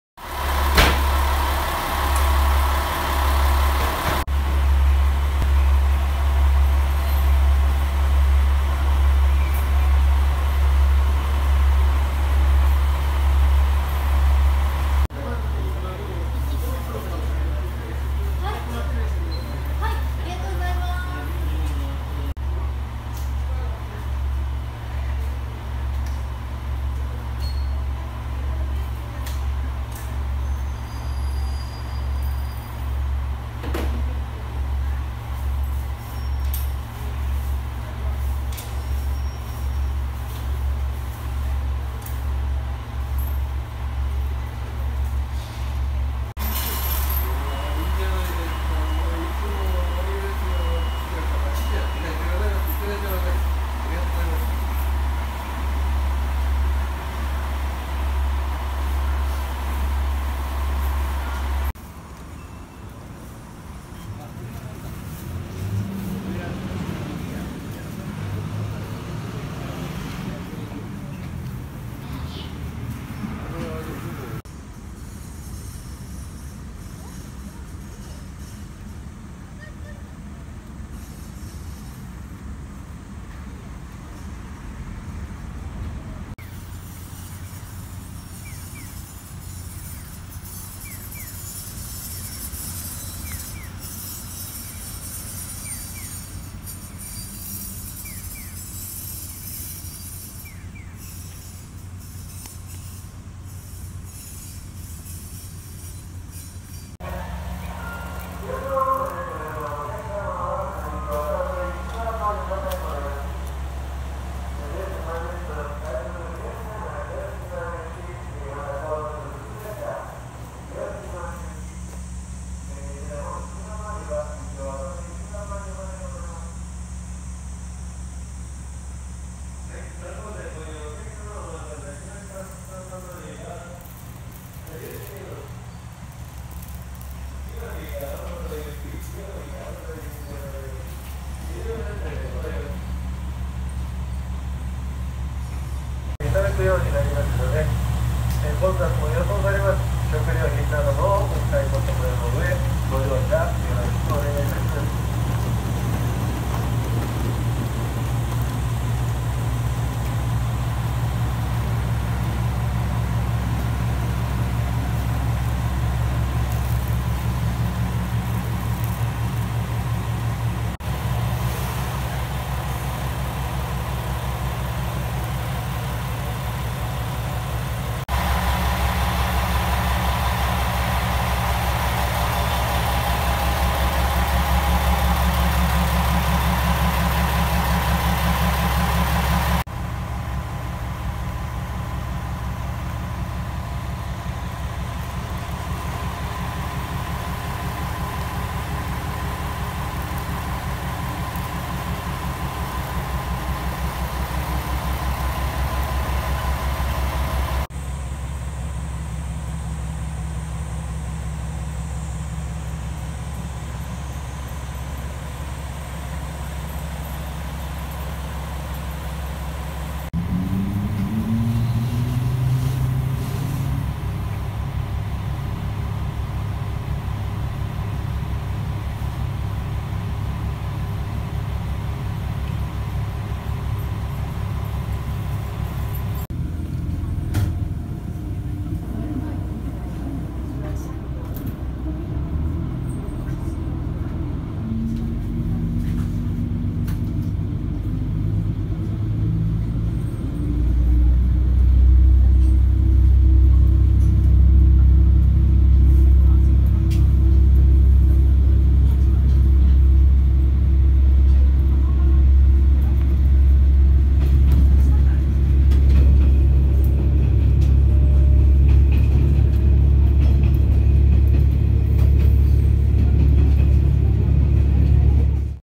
【三江線】三次駅の様子（Miyoshi Station）【芸備線】
訪問：2017年7月25日（火）

広島県北部にある三次駅
芸備線、三江線、福塩線が乗り入れ、交通の要衝となっているが、
来年の2018年春には、三江線の廃線が決まっている。
今回は広島から芸備線で三次駅に行き、そこから三江線へ。
三江線は１日に５本程度しかなく、三次から江津までの直行も存在しない。
しかしもうすぐ廃線という事で、平日を選んだのにも関わらず、けっこうな旅行者でにぎわいました。

鉄道／旅行／駅／芸備線／三江線／福塩線／広島県／三次駅／駅メモ